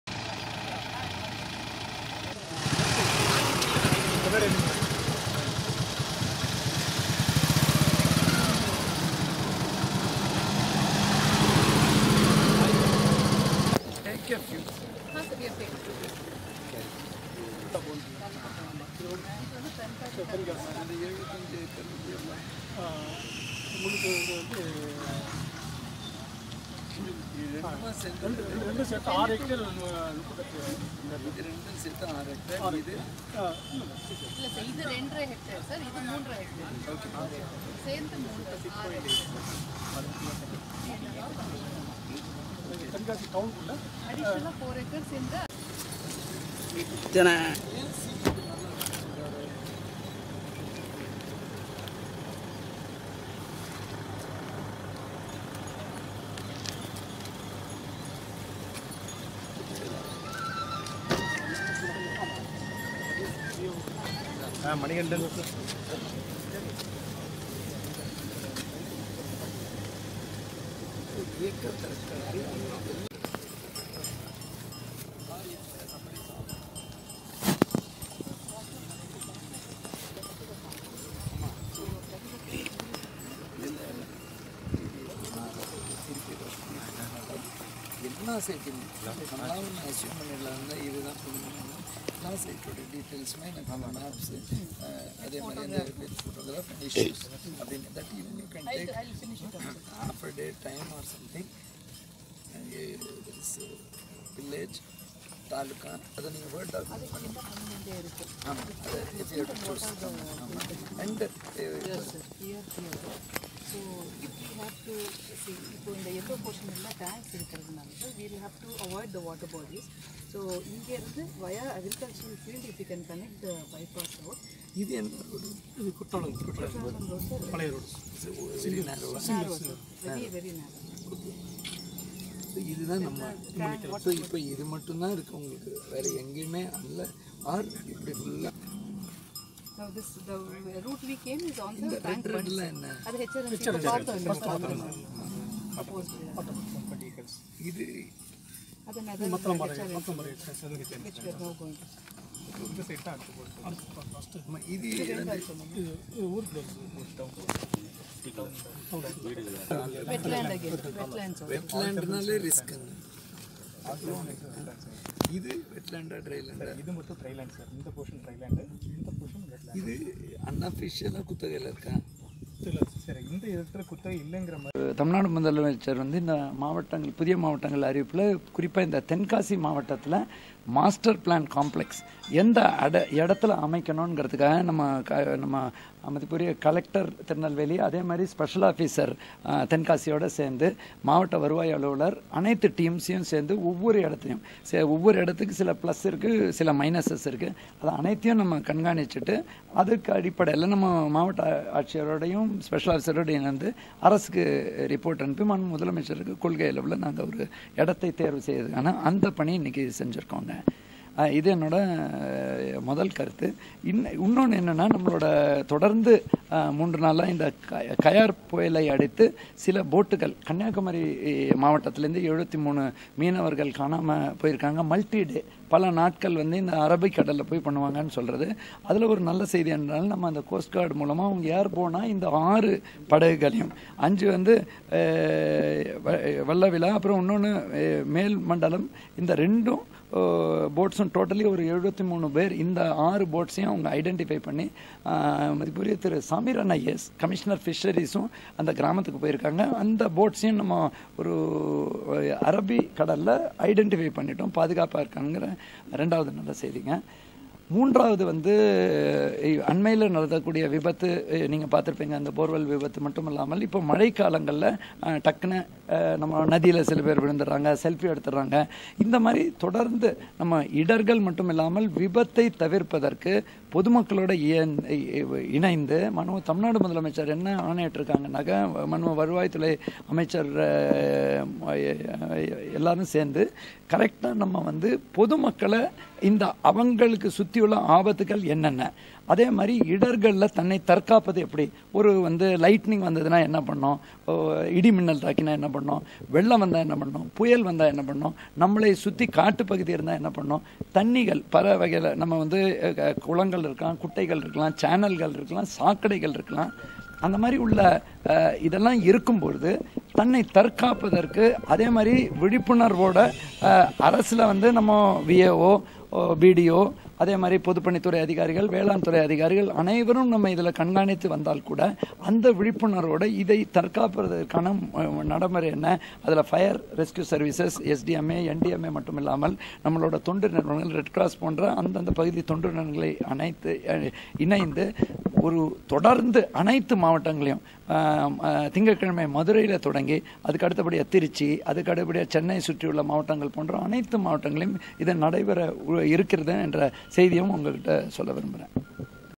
தென்காசி மாவட்ட பிரிவு பணிகள் குறித்து வருவாய் துறை இயக்குநர் ராதாகிருஷ்ணன் ஆய்வு
கன்னியாகுமரியில் காணாமல் போன மீனவர்கள் இருக்கும் இடம் உறுதி செய்யப்பட்டுள்ளது. வருவாய் துறை இயக்குநர் ராதாகிருஷ்ணன் குற்றாலத்தில் பேட்டி.

நெல்லை மாவட்டம் தென்காசி பகுதிகளில் மாவட்டத்தின் புதிய அலுவலகங்கள் அமைய உள்ள இடங்களை வருவாய்த்துறை இயக்குனர் ராதாகிருஷ்ணன் பார்வையிட்டு ஆய்வு மேற்கொண்டார். அதனை தொடர்ந்து செய்தியாளர்களை சந்தித்தார். அப்போது அவர் கூறியதாவது:-

நெல்லை மாவட்டம் தென்காசியை தனி மாவட்டமாக பிரிக்கும் நிகழ்வில் தென்காசி மாவட்டத்தில் தலைமை அலுவலகங்கள் அமைக்கும் இடங்களை பார்வையிட்டுள்ளோம்.  இந்த ஆய்வு அறிக்கையாக சமர்பிக்கப்படும்

தொடர்ந்து, புயல் காரணமாக  கன்னியாகுமரியில் உள்ள 73 மீனவர்கள் மற்றும் சில படகுகள் பல நாட்களாக   காணாமல் போனதாக கூறப்பட்டது. இந்த நிகழ்வில் கடற்படை மூலம்,  வல்லவில்லா  5 மேல் மண்டல பகுதியில் படகுகளும்,73 மீனவர்களும் பாதுகாப்புடன் இருப்பது உறுதி செய்யப்பட்டுள்ளது.

மழைக்காலங்களில் இடர்கள் மற்றும் விபத்தை தவிர்க்க தமிழக முதல்வரின் ஆணையின் படி பொதுமக்கள் அவர்களை சுற்றி உள்ள ஆபத்துக்களை அறிய மற்றும் தற்காத்துக்கொள்ள விழிப்புணர்வு ஏற்படுத்தபட உள்ளது. தற்பாதுகாப்பு குறித்த விழிப்புணர்வுகளை ஏற்படுத்த தொண்டு நிறுவனங்களுடன் இணைந்து அனைத்து மாவட்டங்களிலும் தொடர்ந்து விழிப்புணர்வு ஏற்படுத்தப்படும்.

இந்த விழிப்புணர்வு முதற்கட்டமாக  திங்கள் கிழமை மதுரையில் இருந்து தொடங்கப்பட உள்ளது. அதனை தொடர்ந்து மாநிலம் முழுவதும் விழிப்புணர்வு நிகழ்ச்சிகள் நடைபெறும் . இவ்வாறு அவர் தெரிவித்தார்.

பேட்டியின் போது மாவட்ட ஆட்சியர் ஷில்பா பிரபாகர் சதீஷ், மாவட்ட வருவாய் அலுவலர்  முத்துராமலிங்கம் தென்காசி கோட்டாட்சியர் உள்ளிட்ட முக்கிய அதிகாரிகள் இருந்தனர்.